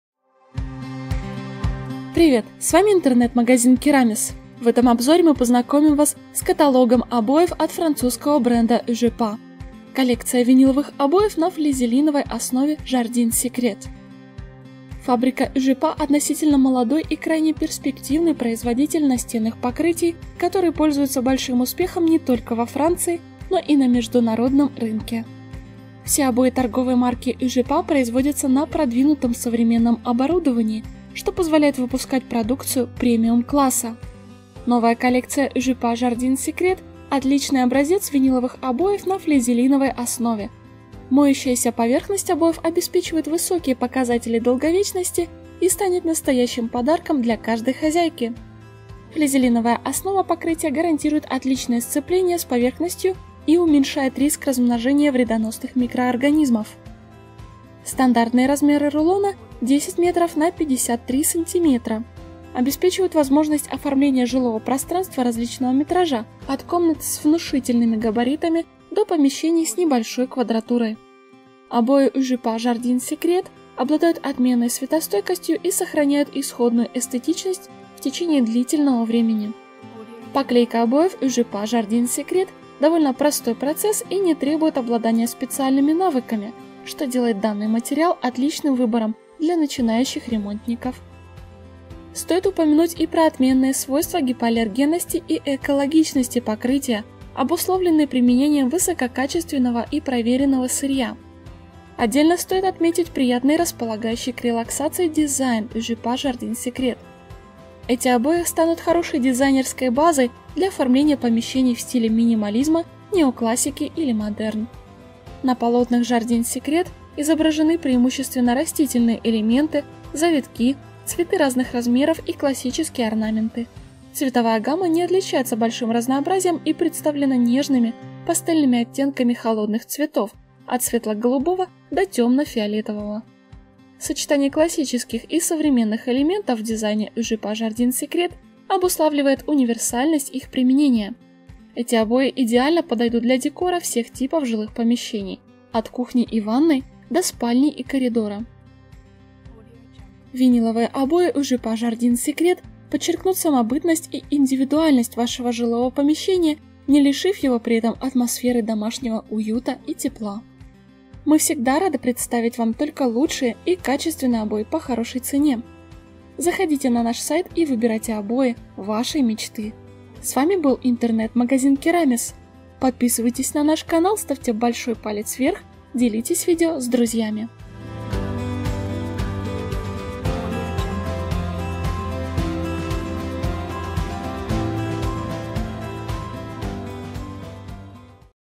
Пикантные виниловые обои из Франции Ugepa Jardin Secret
Все обои торговой марки Ugepa производятся на продвинутом современном оборудовании, что позволяет выпускать продукцию премиум-класса.
Виниловые обои Ugepa  Jardin Secret подчеркнут самобытность и индивидуальность вашего жилого помещения, не лишив его при этом атмосферы домашнего уюта и тепла.

Краткие характеристики:
Страна
Производитель
Коллекция: Jardin Secret
Ширина рулона: 0.53 м
Длина рулона: 10,05 м.п.